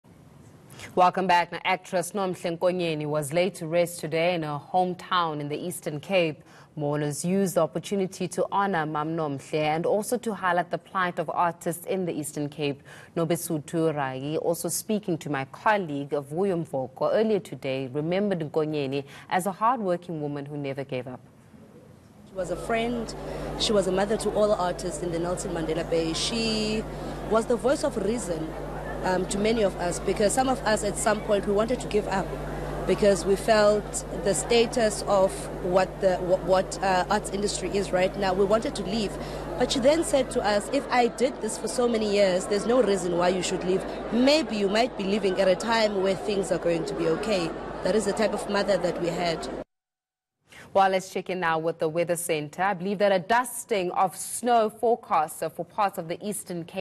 Nomhle Nkonyeni laid to rest
Actress Nomhle Nkonyeni was laid to rest today in her home town in the Eastern Cape. Mourners used the opportunity to honour mam' Nomhle and also to highlight the plight of artists in the Eastern Cape. Courtesy